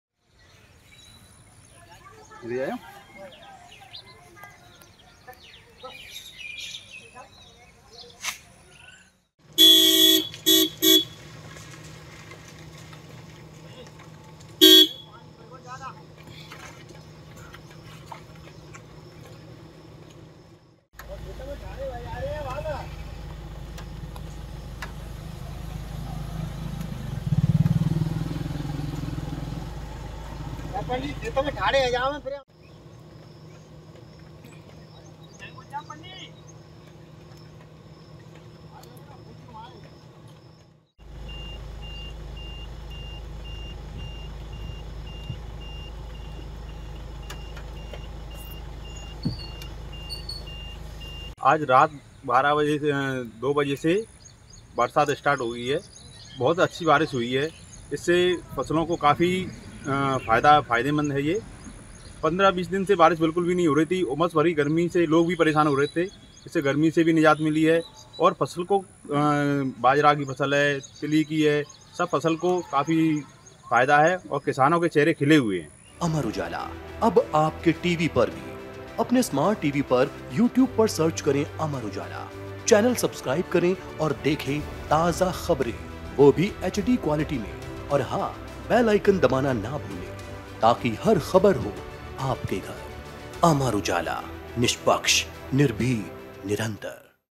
धौलपुर जिले में झमाझम बारिश का दौर शुरू, खिल उठे किसानों के चेहरे | Rajasthan Weather | Dholpur Rain
धौलपुर जिले में झमाझम बारिश का दौर शुरू हो गया है. बारिश के बाद किसानों के चेहरे खिल उठे हैं.